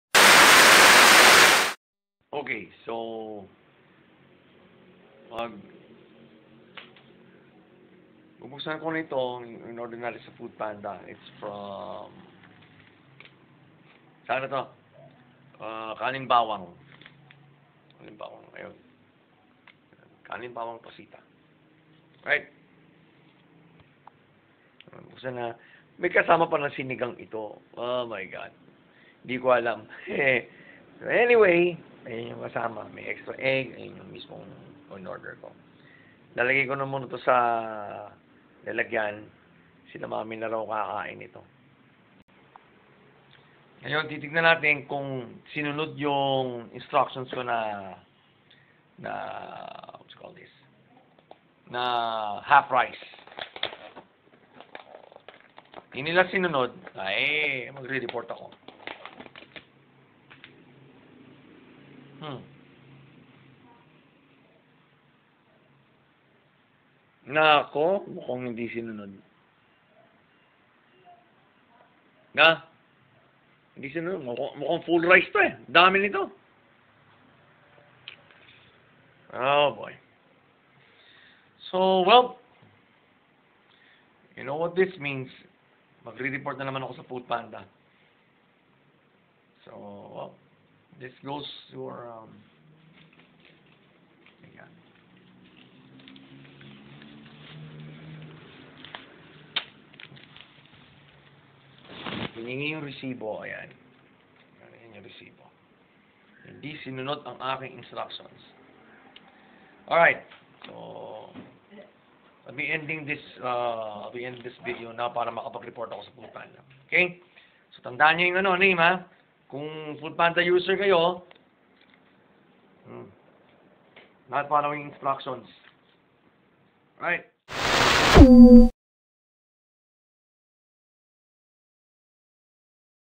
Reporting Kanin Bawang - Pacita | Foodpanda Merchant Review
My first-ever merchant review. After all, we've been having our food delivered since this pandemic started.

You might also want to consider using this app, too (for cashback on your orders) 👉🏽